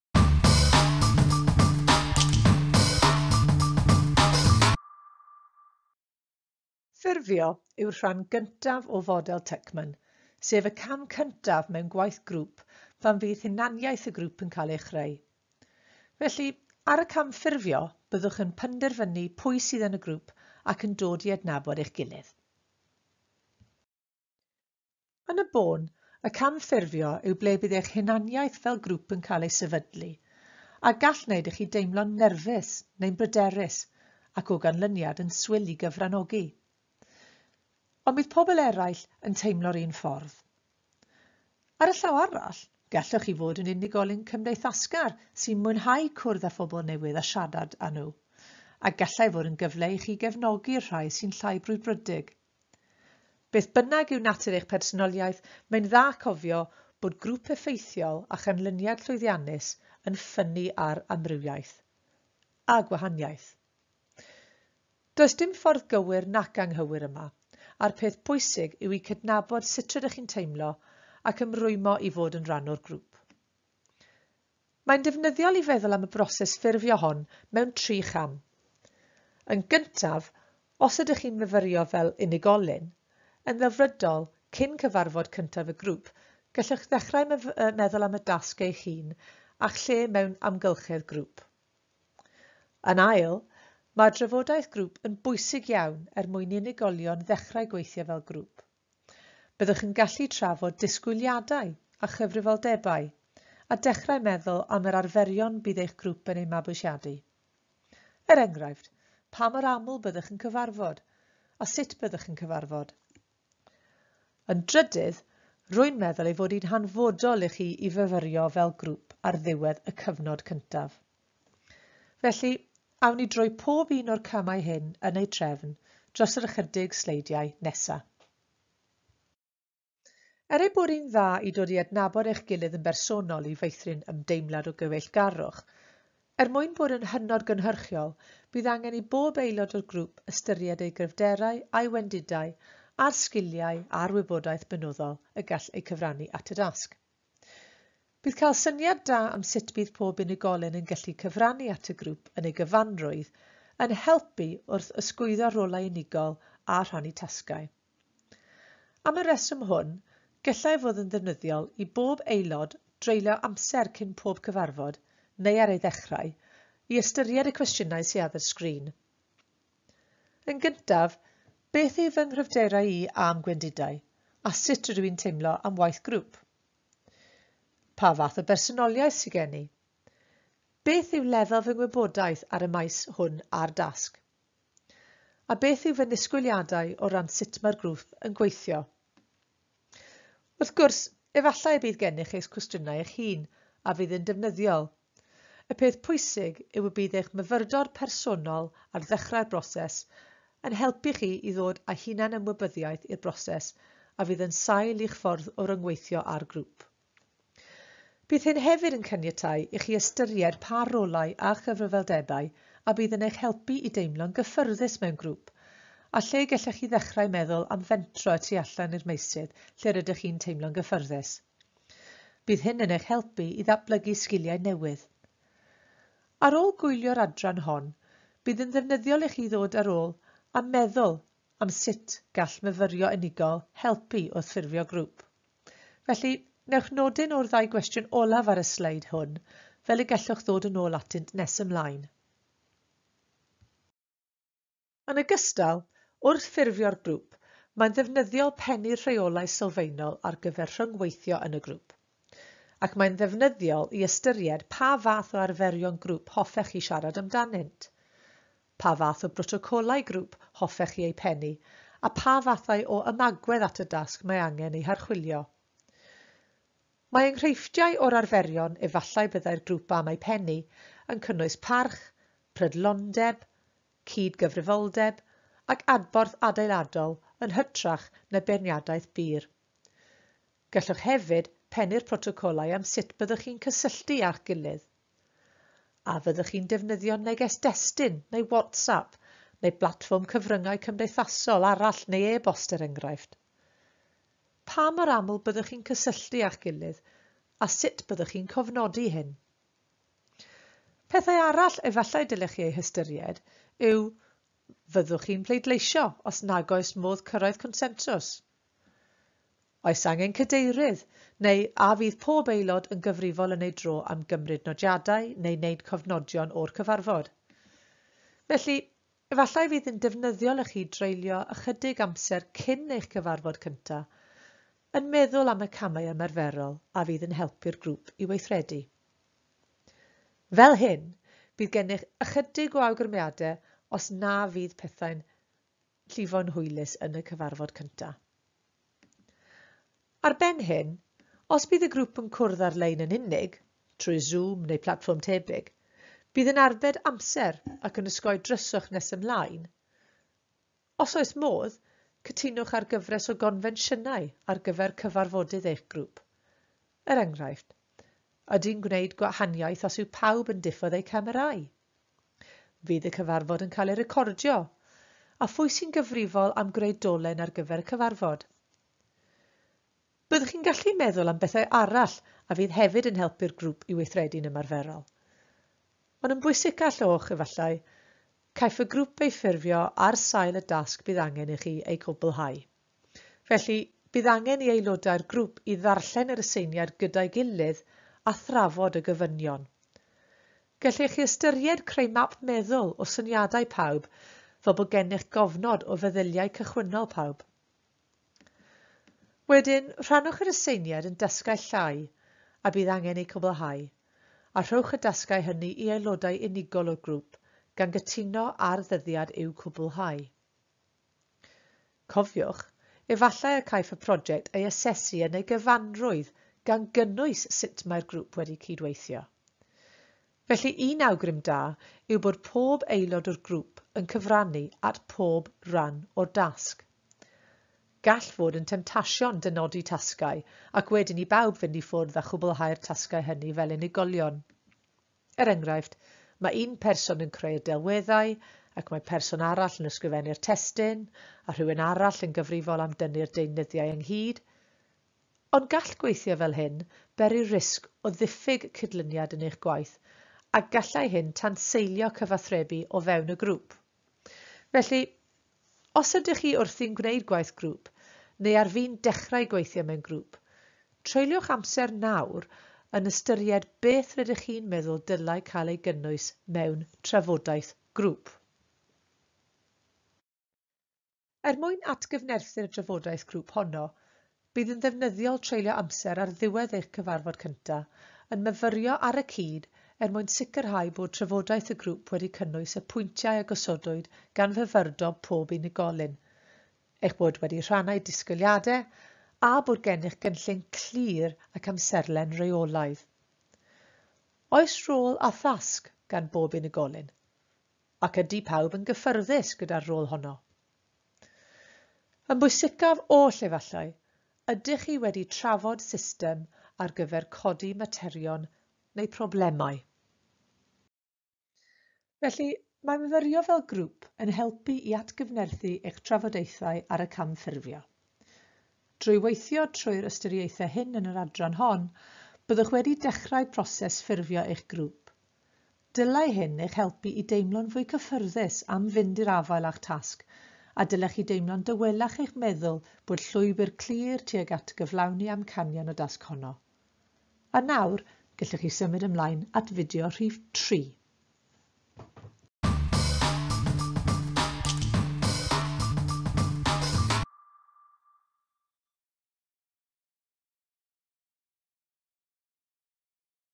Gweithio mewn Grwpiau (Rhan 2) - Ffurfio
Mae’r fideo hwn yn eich annog chi i feddwl am gam cyntaf creu grŵp. Dyma pryd byddwch chi’n meddwl am eich swyddogaeth chi fel unigolyn yn y grŵp, gan ofyn i chi archwilio eich ofnau a’ch gobeithion ar gyfer datblygu grŵp llwyddiannus.